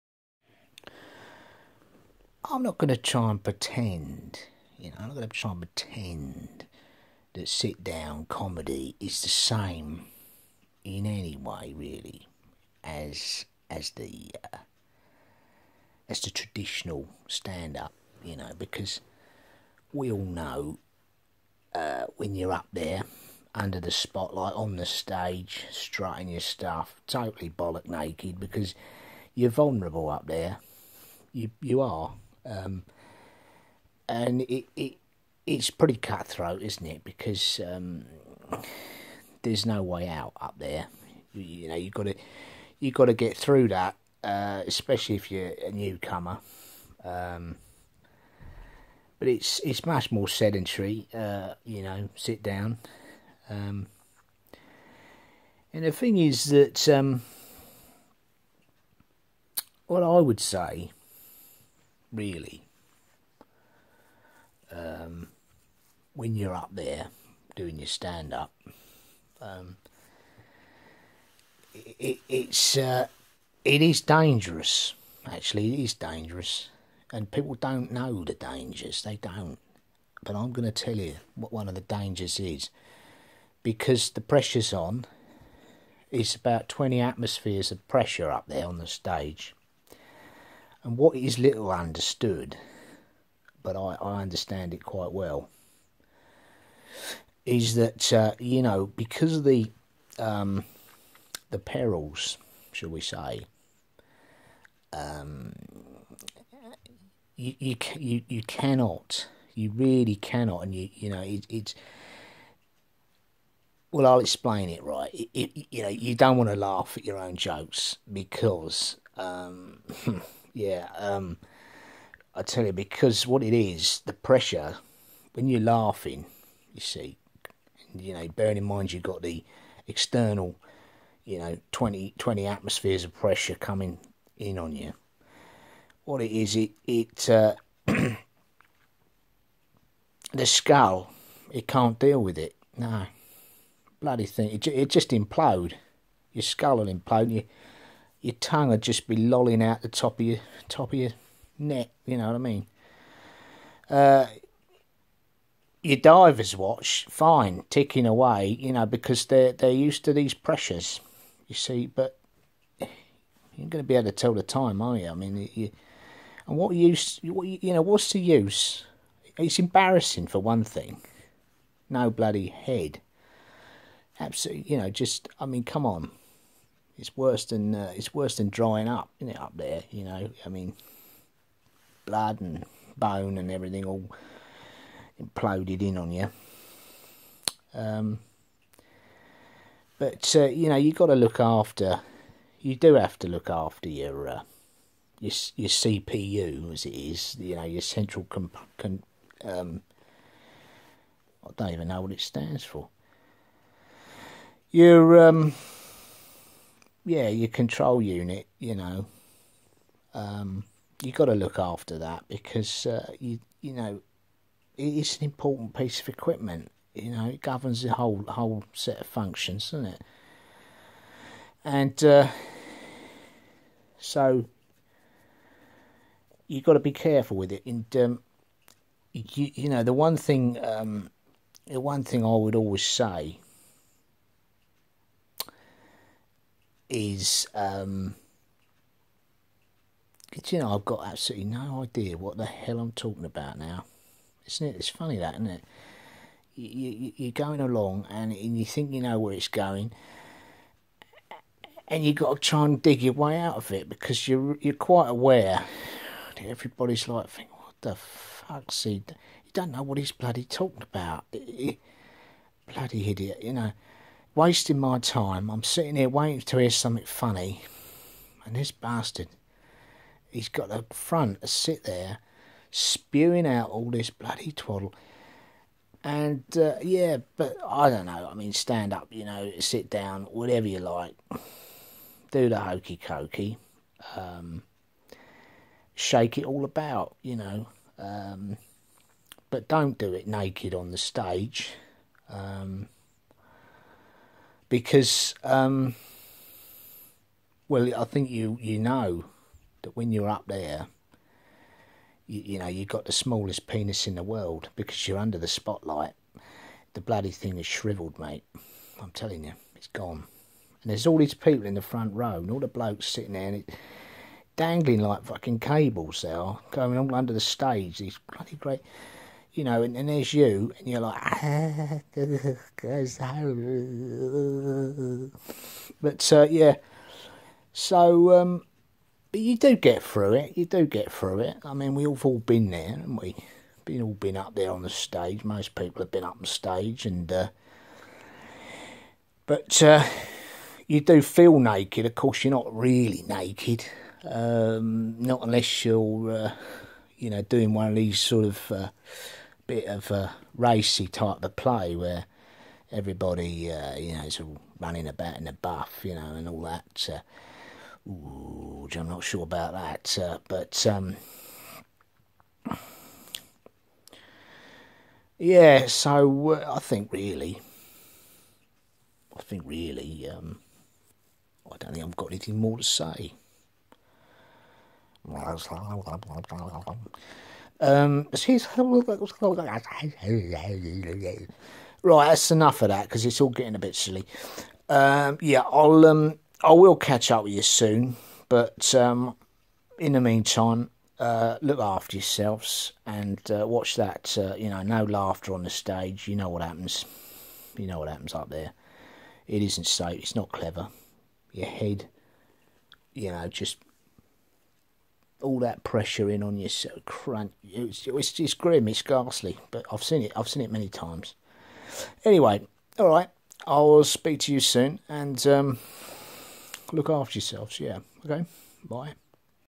Stand-up under the spotlight - the smallest penis in the world...
Nipsy explains the perils of stand-up - feeling naked on stage and that smallest-penis-in-the-world feeling...